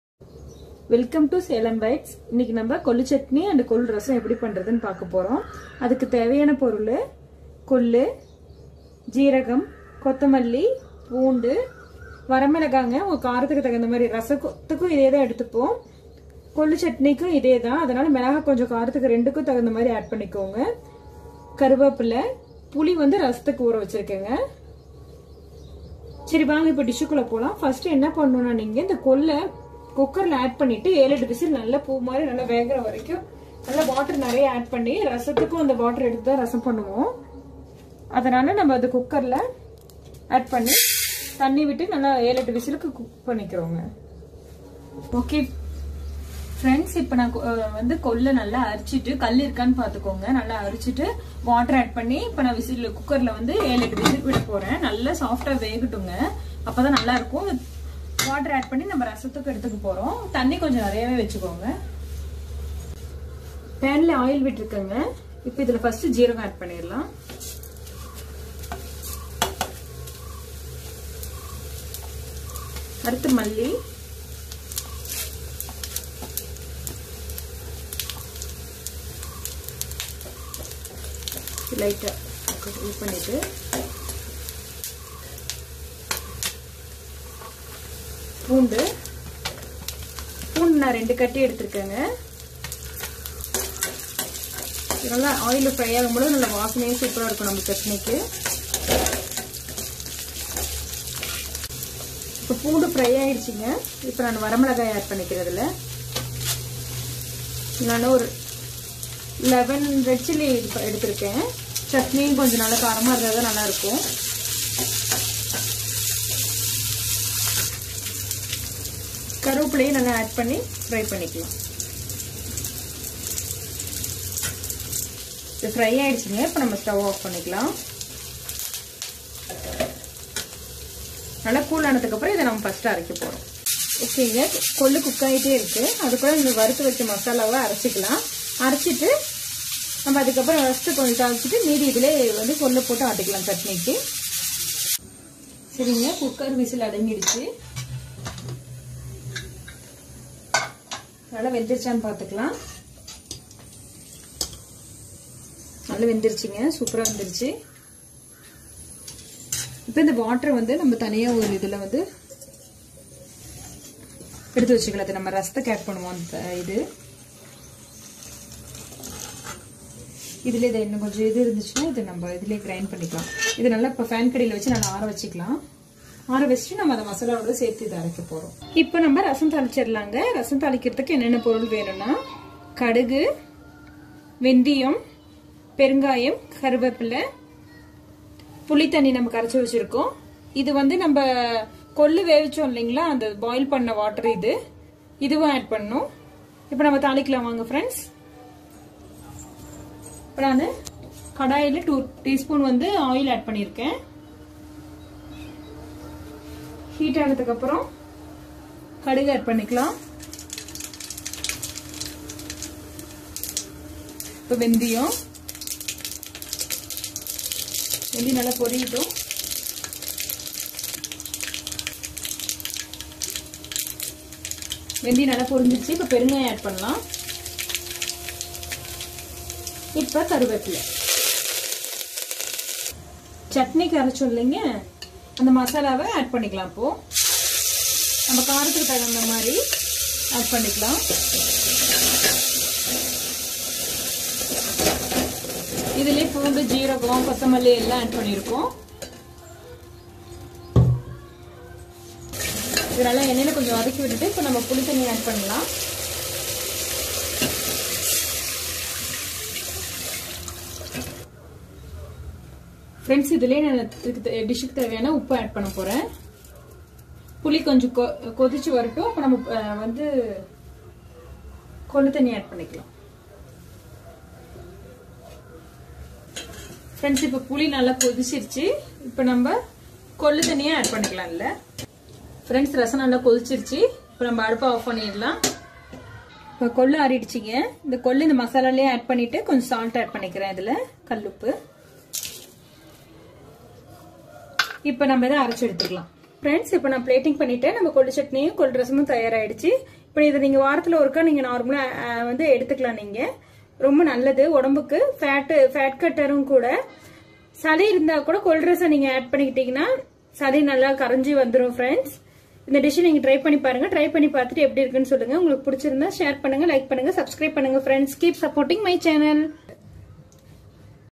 Kollu Thuvaiyal&Rasam/Horsegram Chutney&Rasam/ கொள்ளு துவையல் & ரசம்
Ingredients:
Horsegram dal-250g
Oil-2 tbsp
mustard-1/2 tsp
jeera-1 tsp
Corainder seeds-1 tsp
Redchilly-10
Curry leaves
Garlic-1/4 cup
Tamarind-1/2 lemon size
Preparation:
              Boil the horsegram in the presure cooker with 8 whistles.Heat oil in the pan add jeera,corainder seeds,garlic,redchilly,curry leaves and fry well until the aroma comes.Once it cools down grind it properly until the smoothless comes.Take some mixture and keep aside for rasam.Add the boiled and cooked horsegram with that mixture in the mixer and grind well and add salt.Transfer the chutney with the bowl and garnish it with with small onions.Serve it with hot rice.
         Now we make rasam,heat oil in the the pan add mustard, fenugreek, asafoetida powder,curry leaves and fry well.Then add grinded mixture,tamarind water and horsewater water into it.Wait until the rasam boils.Now our rasam is ready serve it with hot rice.